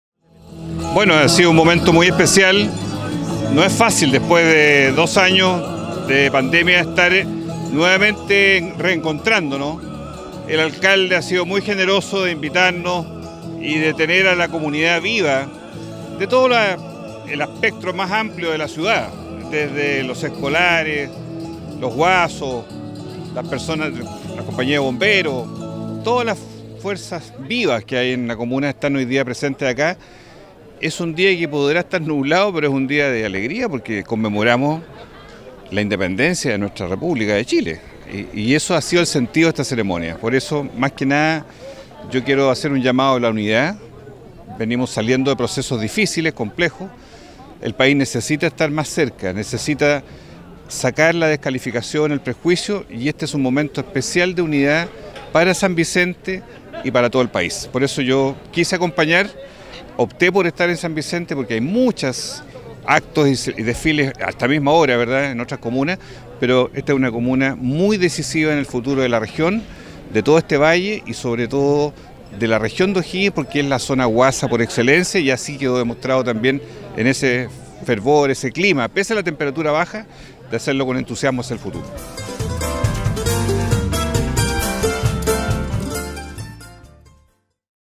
FIESTAS PATRIAS 2022
El senador de la República Juan Luis Castro participó, del desfile de Fiestas Patrias en nuestra comuna, donde destacó que San Vicente es una de las más importantes zonas huasas del país. Felicitó a los participantes y  al municipio, a través del Alcalde Jaime González,  por realizar tan destacada actividad.